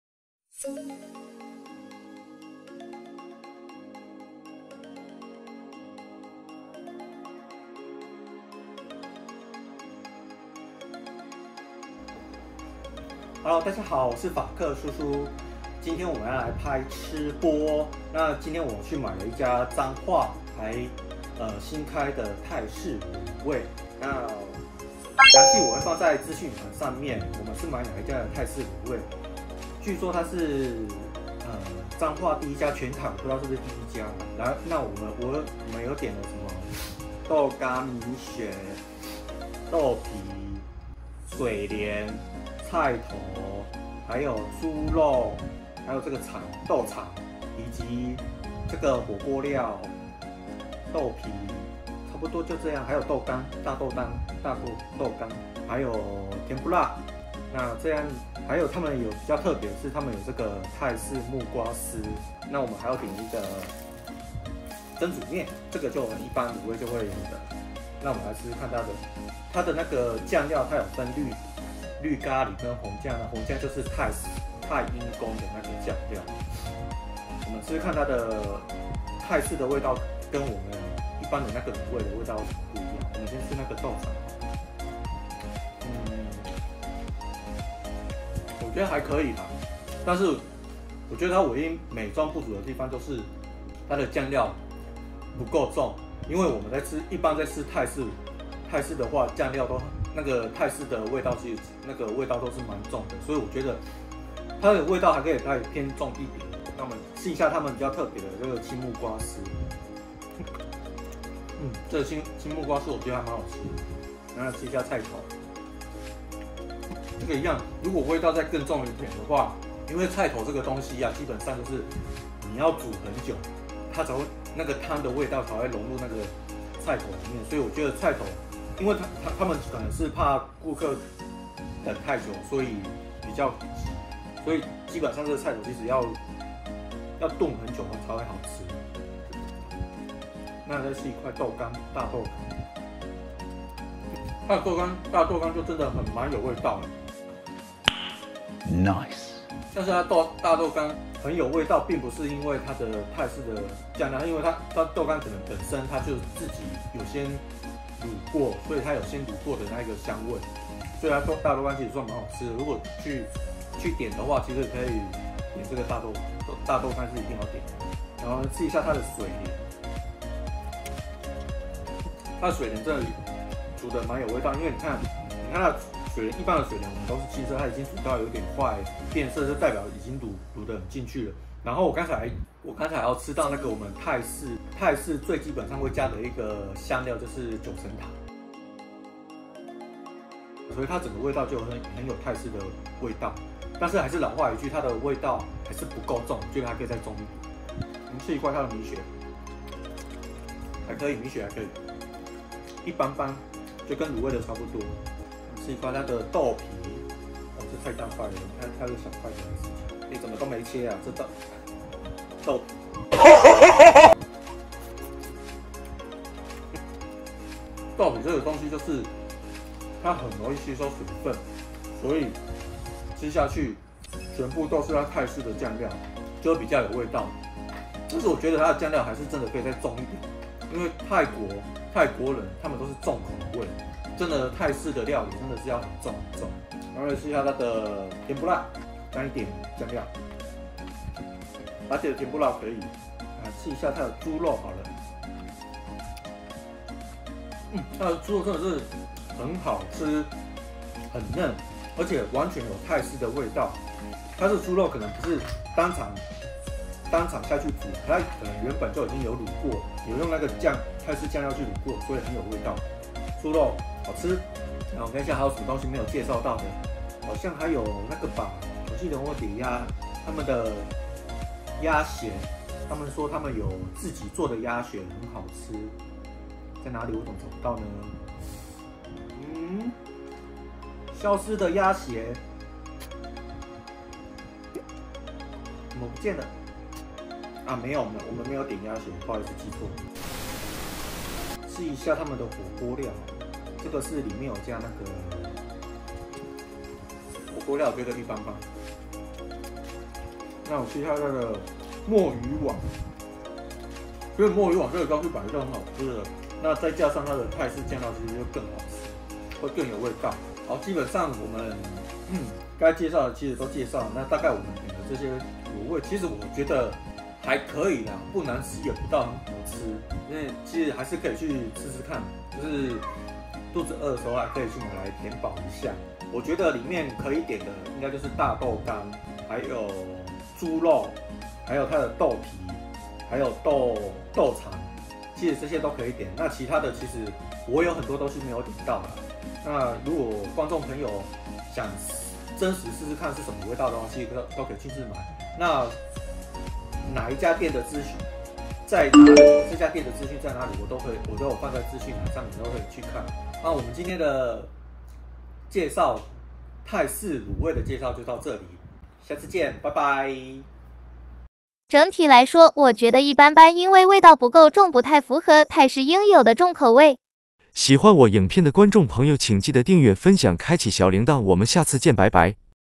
太泰滷味 Super Thai 試吃心得分享
太泰滷味 Super Thai (彰化華山壹號店)
門市位置：500彰化縣彰化市華山路1號

廠商合作請 IG發訊息告知
IG追蹤法藍客：keyboardfrank
FB追蹤粉專：artistfranksu